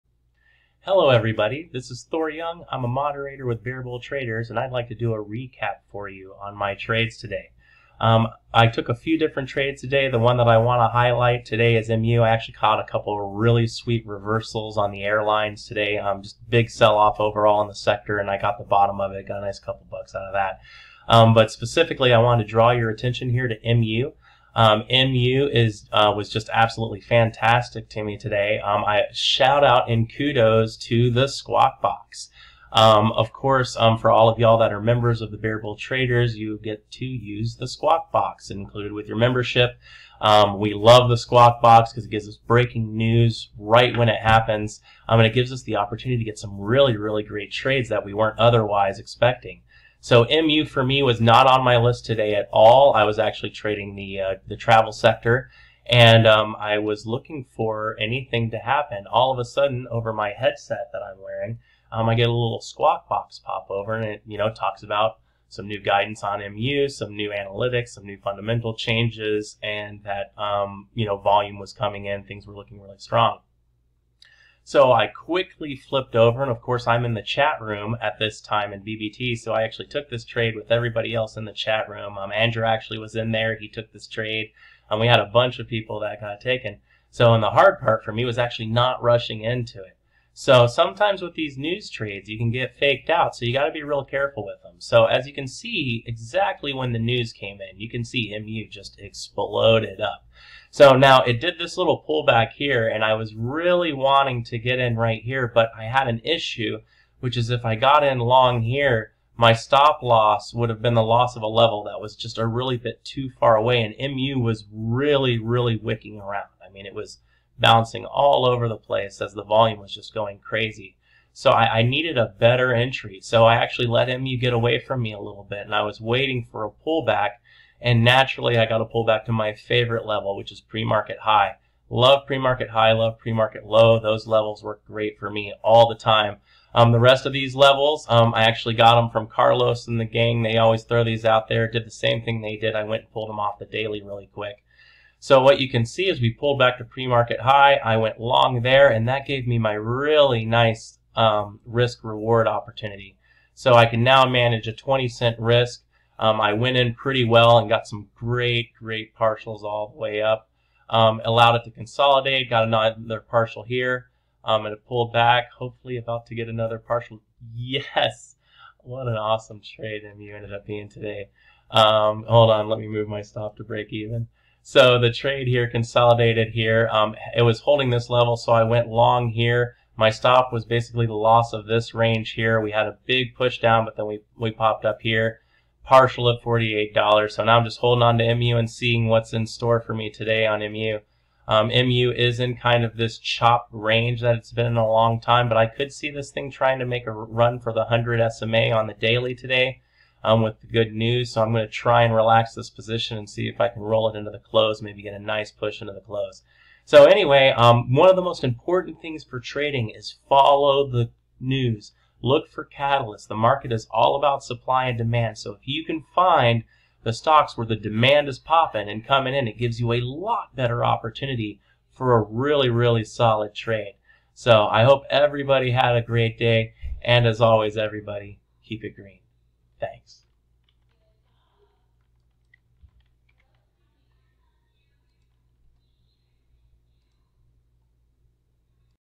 Why you need a good news service for Day Trading! Live $MU trade after Benzinga Squawk call out.
🚨 Check Book "How to Day Trade for a Living”